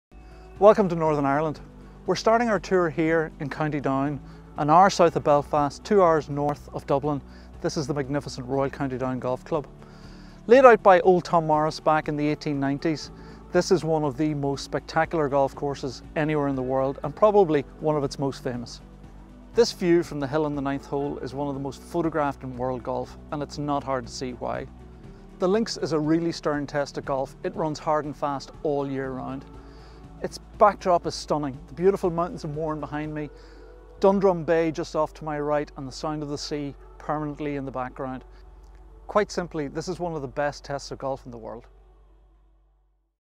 Royal County Down Golf Club
Take a brief look at Royal County Down Golf Club, globally recognised as one of the best golf courses in the world.